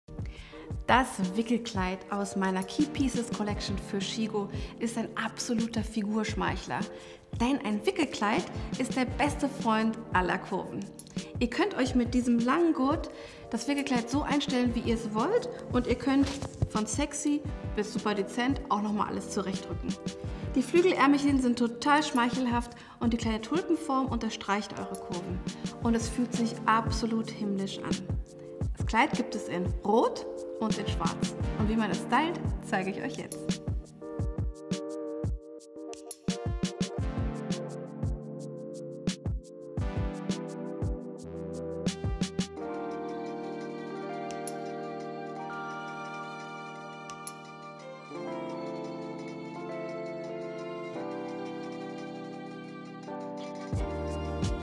sheego by Miyabi Kawai - Stylingtipps - Wickelkleid
"Wir alle brauchen Styles, die man ohne nachzudenken anziehen kann. Ein knielanges, schlichtes schwarzes Kleid aus bequemem Viskosejersey zum Beispiel ist die perfekte Basis für viele Anlässe. Aber auch in Rot hat das Kleid seinen Platz auf der Liste meiner Key-Pieces absolut verdient. Mit schicken Pumps macht es auf jeder Sommerhochzeit eine tolle Figur, mit weißen Sneakern begleitet es mich spontan auf einen Cappuccino ins nächste Straßencafé." by Miyabi Kawai

Entdecke jetzt Trendmode in großen Größen von Miyabi Kawai bei sheego

Schau dir weitere Videos in unserer exklusiven sheego by Miyabi Kawai Playlist in unserem Kanal an